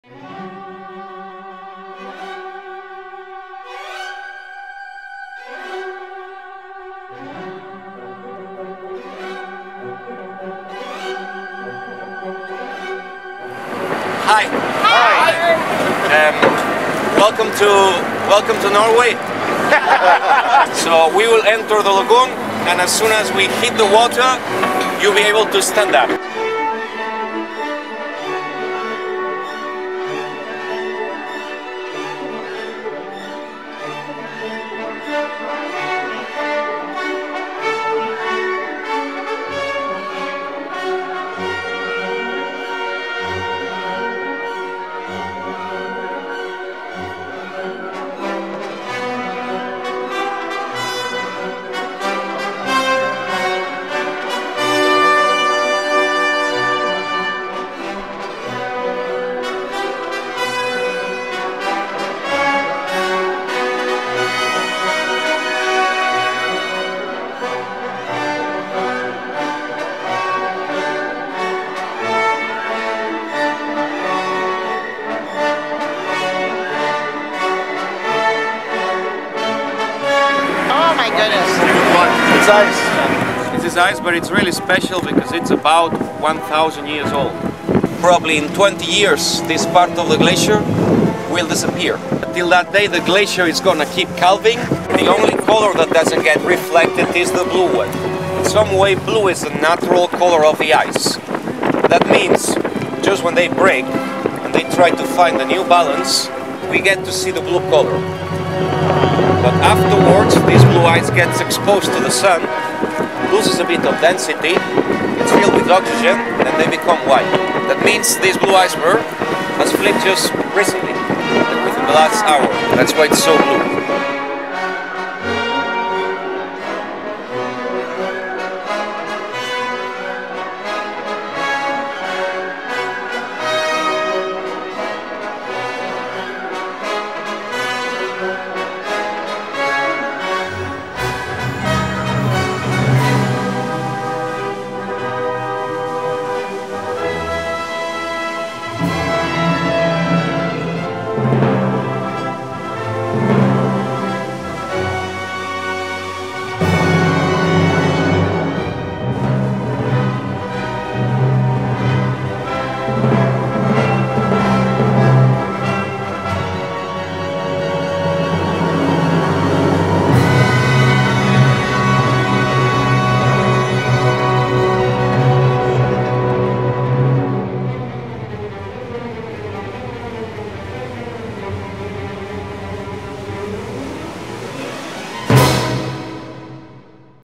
A boat ride through the Jökulsárlón Glacier Lagoon, Iceland.
An amphibious boat takes you through the icebergs of the Jökulsárlón Glacier Lagoon at the southern end of Iceland.